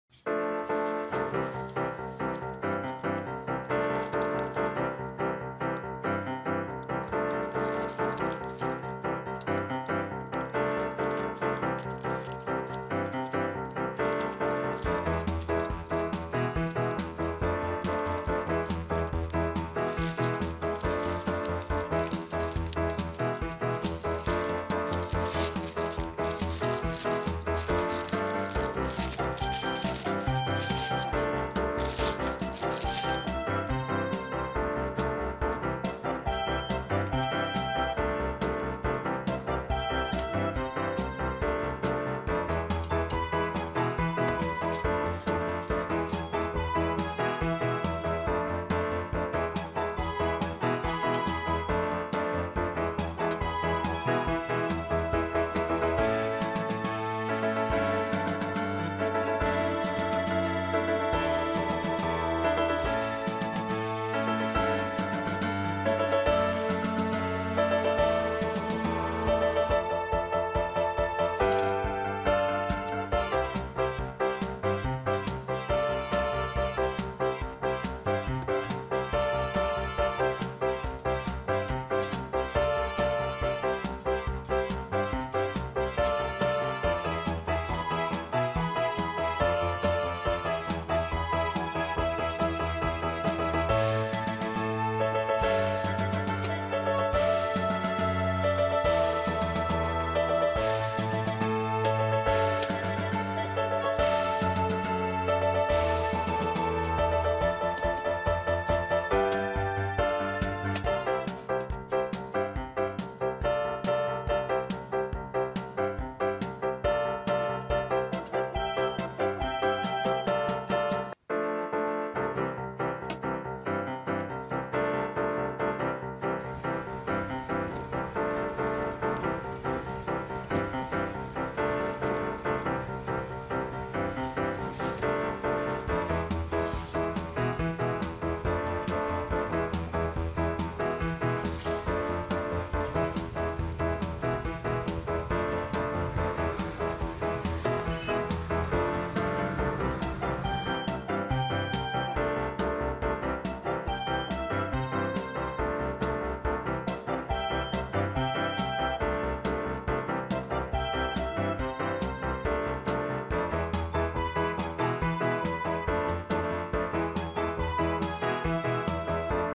Unboxing TOYS Review/Demos - Elsa and Frozen Disney metal case piggy bank with Olaf safe guarding
Grandma toystore brings to you the unboxing demo/review of Elsa and Frozen Disney metal case piggy bank with Olaf safe guarding.  Your kids will definitely save up with this piggy bank.